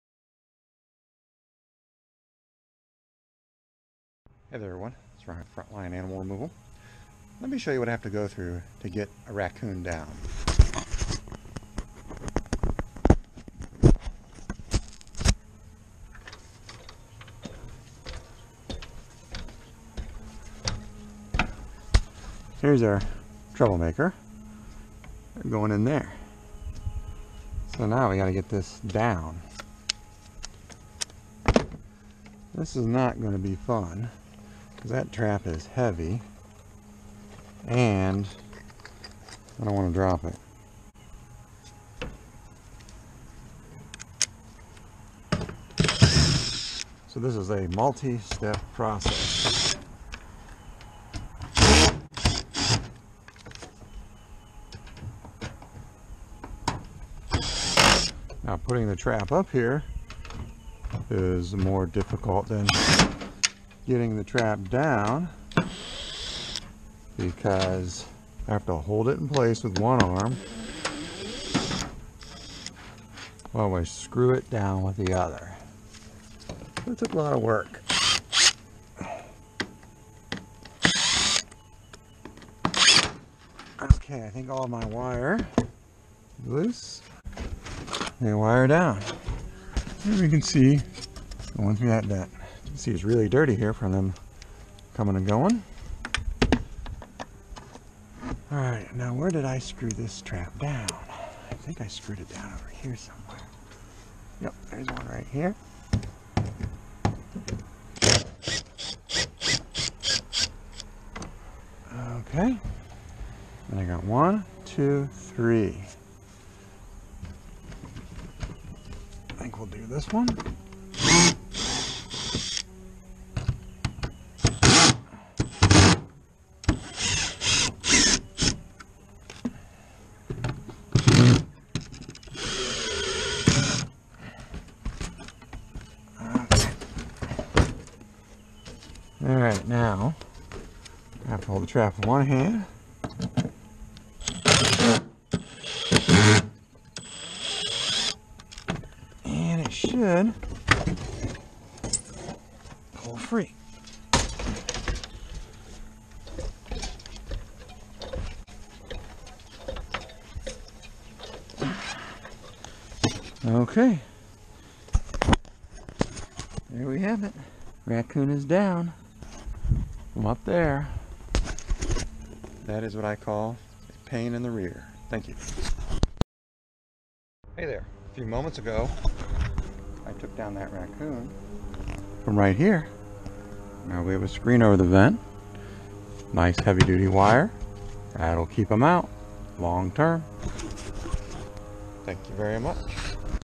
Gable Vent Raccoon : Getting Trap Down & Installing a Screen
This raccoon was accessing an attic at the gable vent.  She was captured in a set that worked whether she was moving out or coming in.

With no babies present yet, the vent is sealed with a screen.